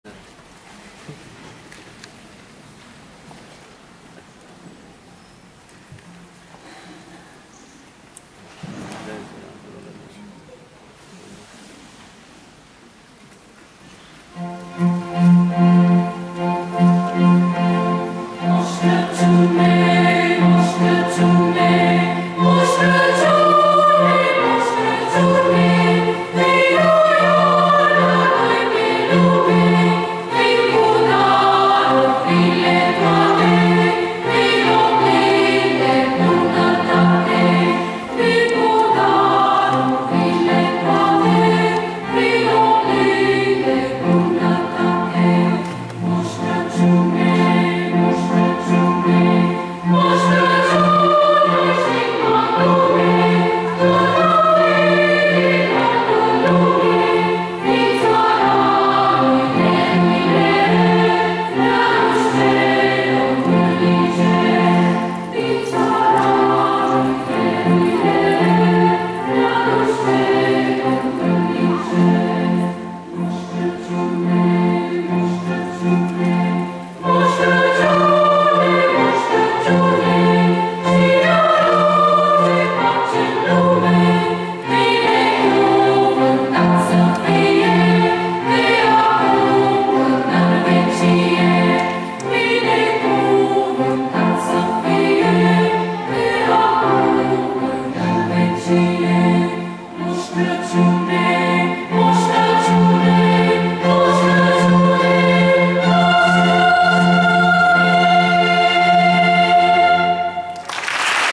Corul de Copii al Operei Romane - Colind
În data de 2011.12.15, la Universitatea din București (Aula Magna a Palatului Facultății de Drept) a avut loc tradiționalul concert de Crăciun.

Au interpretat corul și orchestra Universității din Bucuresti, dirijor Vasile CORJOS, și Corul de copii al Operei Naționale București.

Soliste: Smaranda DRĂGHICI, Elena SERSEA, Mihai PICU, Valentina NĂSTASE, Mihai GROMAŢCHI.